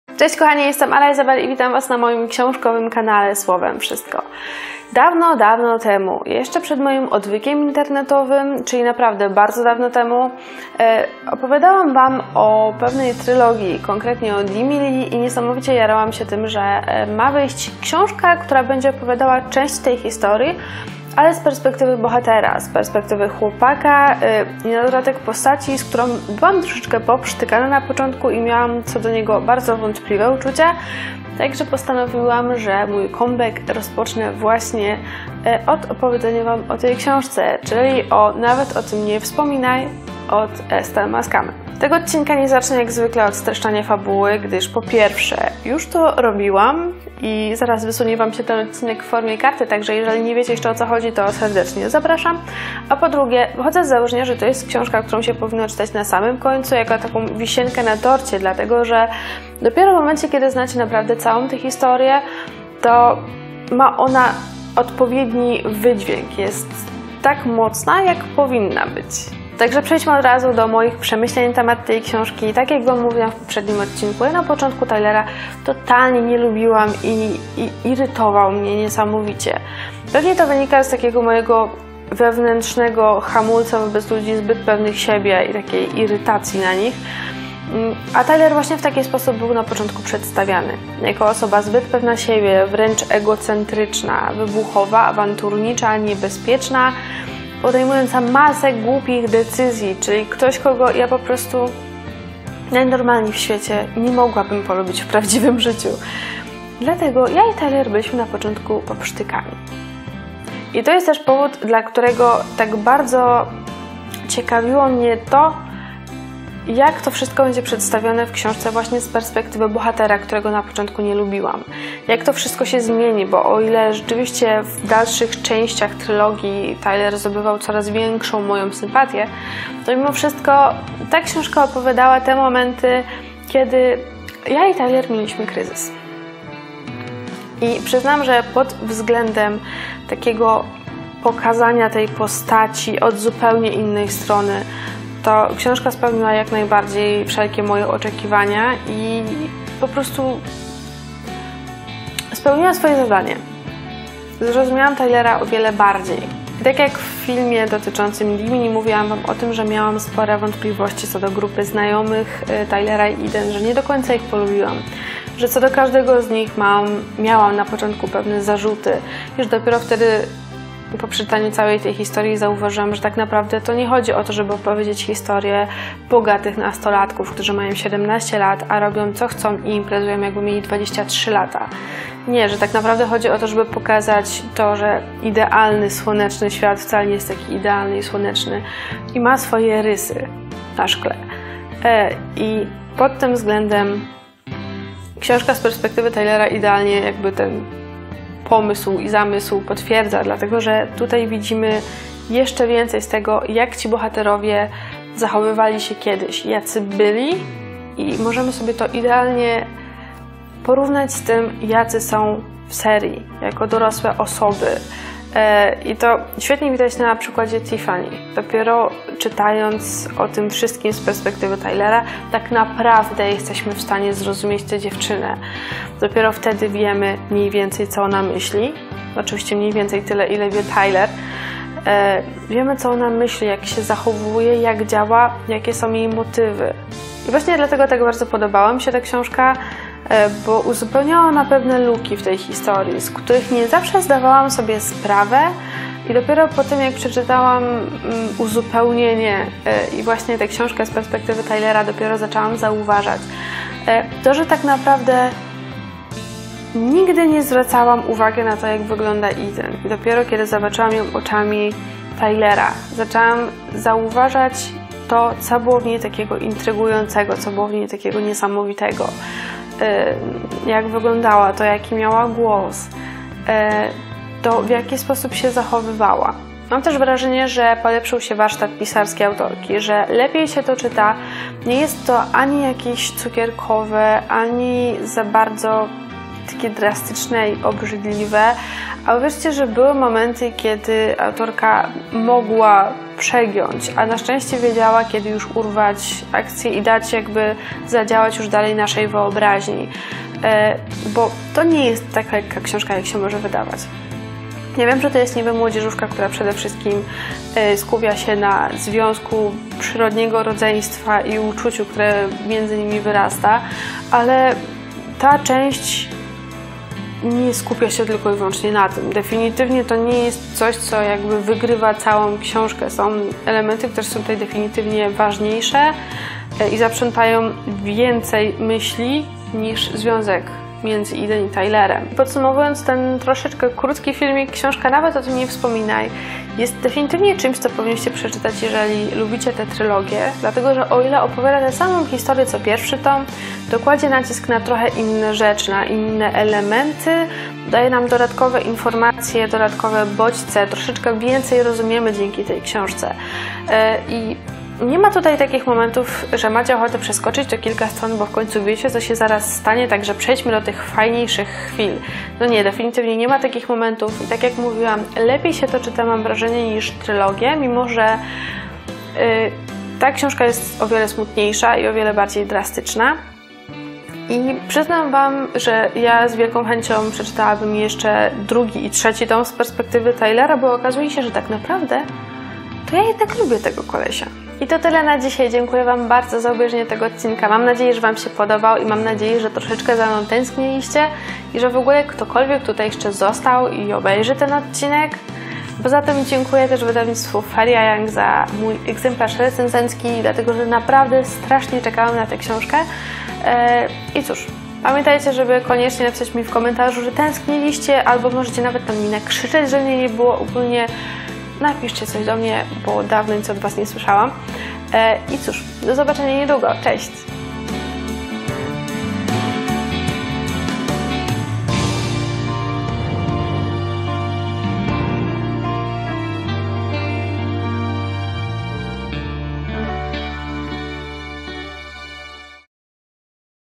Nawet o tym nie wspominaj - Estelle Maskame, czyli o co chodzi temu Tylerowi?
Zapraszam na filmik o czwartym tomie DIMILY, czyli Nawet o tym nie wspominaj od Estelle Maskame i wydawnictwa Feeria.
📘  Jeśli filmik Ci się podobał, zostaw po sobie 👍